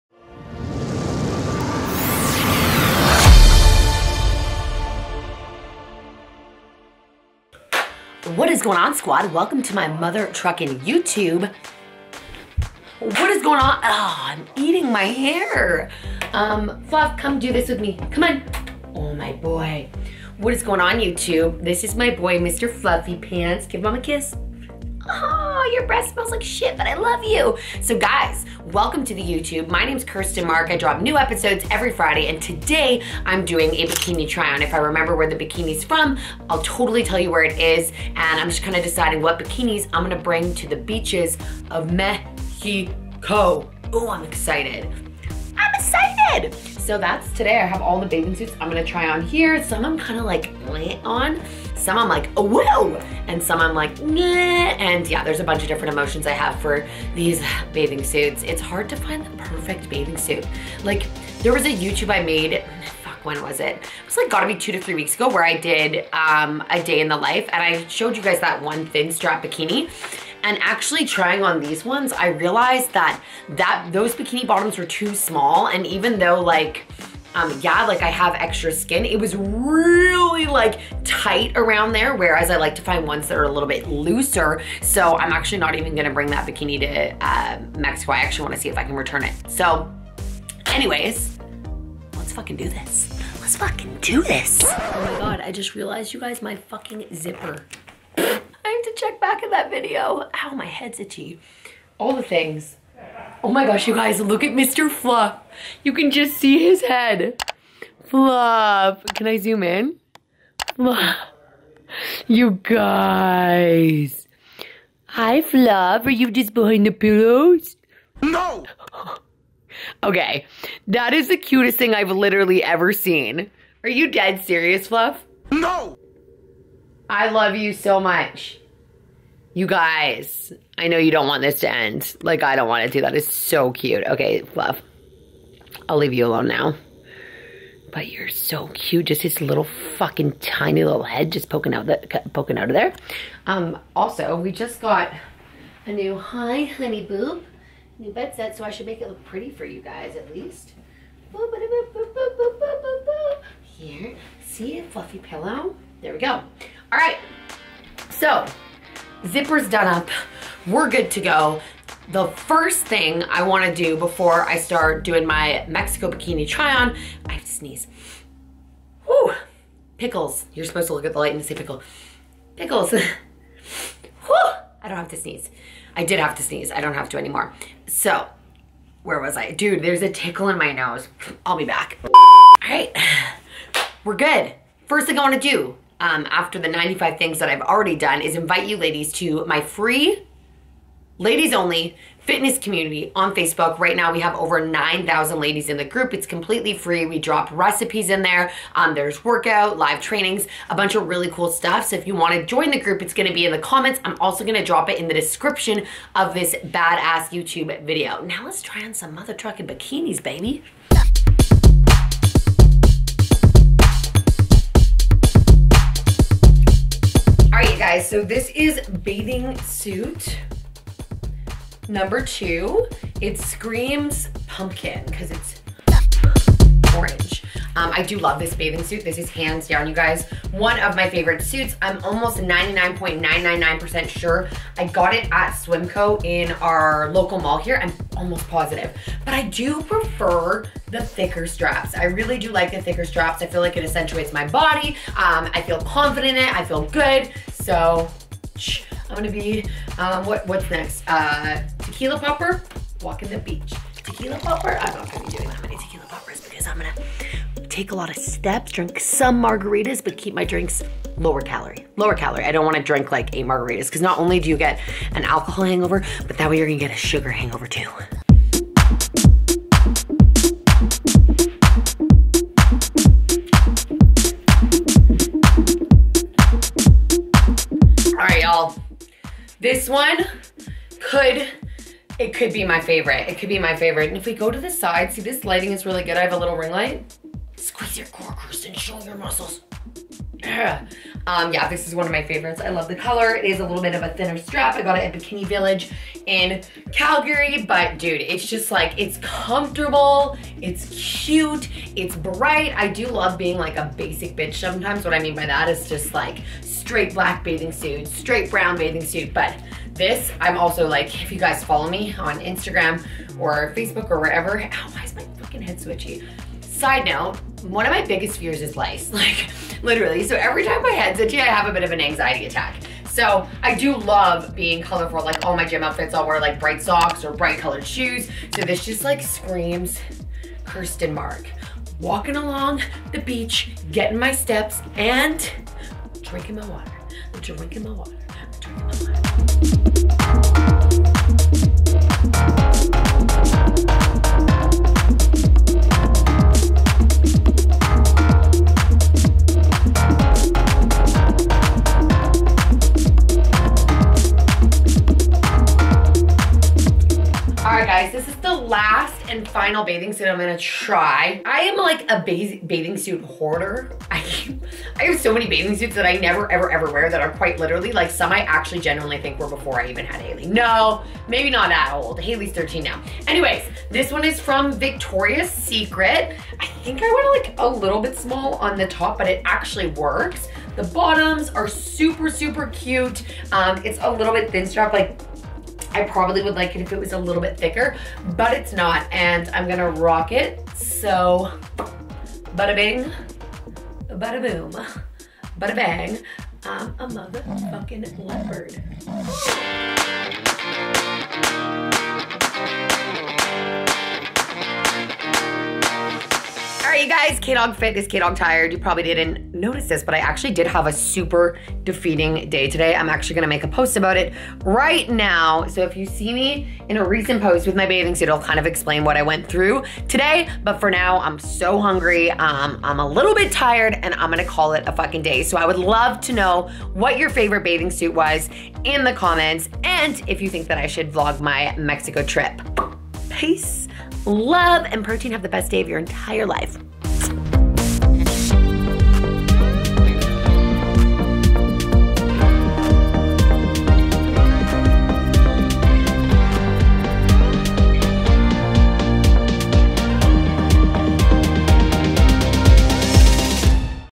MEXICO BIKINI TRY ON [AND REVIEW]
Your girl is off to Mexico so of course, a bikini try was a MUST! What bikini do you like best?